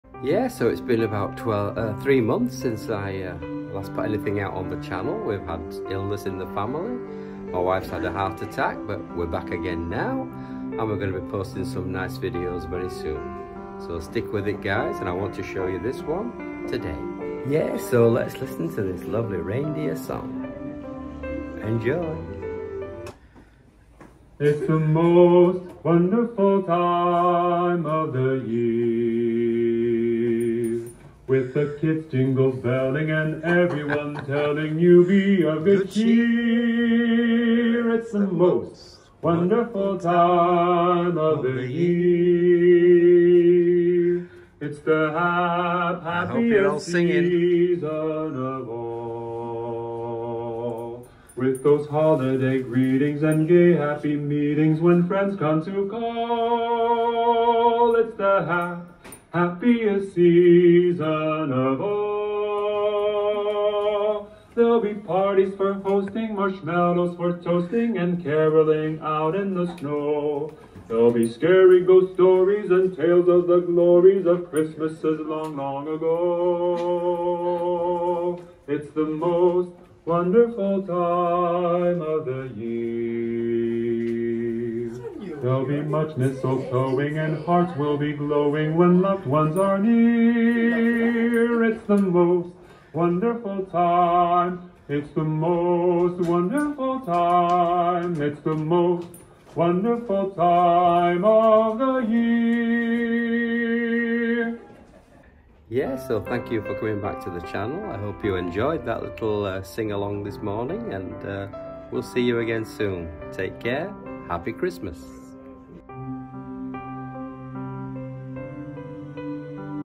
Pops is back for Christmas HD 1080p
Get in the Christmas Spirit, and enjoy a nice singalong. Happy Christmas everyone.